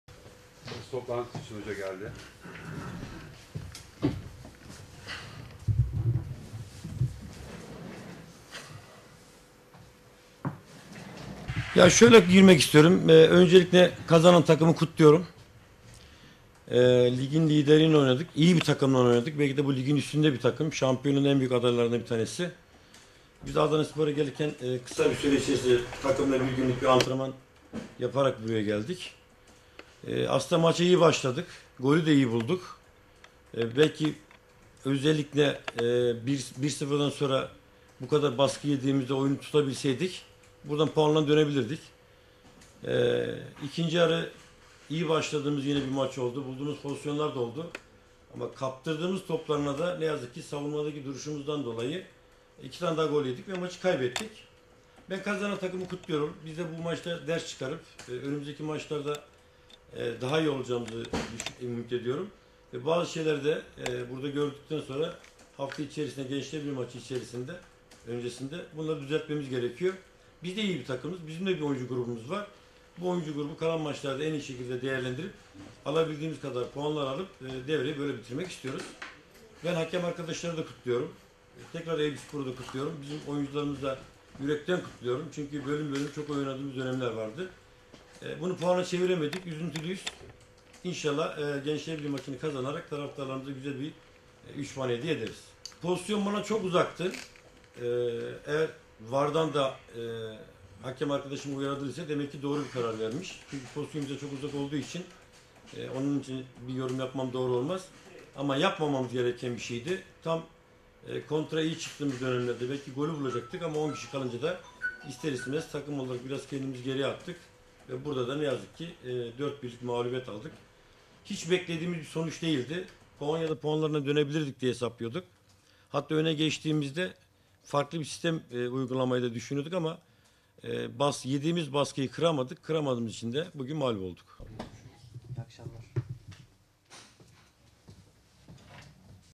Mustafa Kaplan: Eyüpspor, ligin üstende bir...
Adanaspor Teknik Direktörü Mustafa Kaplan, savunma hataları sebebiyle istemedikleri bir mağlubiyet aldıklarını söyledi. Rakiplerinin şampiyonluğun en büyük adaylarından biri olduğunu da ifade edem Kaplan, "Eyüpspor, bu ligin üstende bir kadroya sahip" dedi.